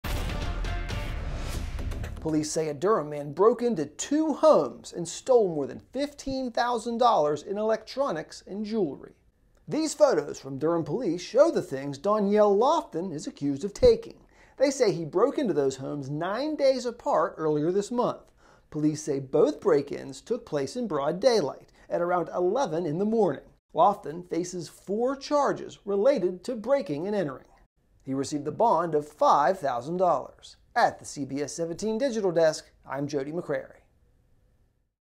Durham man charged with stealing $15,000 in jewelry, electronics during 2 break-ins
Police have accused a Durham man of breaking into two homes in broad daylight and stealing $15,000 worth of items including electronics and jewelry. Durham police on Friday say 45-year-old Donyell Lofton faces two charges of breaking and entering and two counts of larceny after breaking and entering.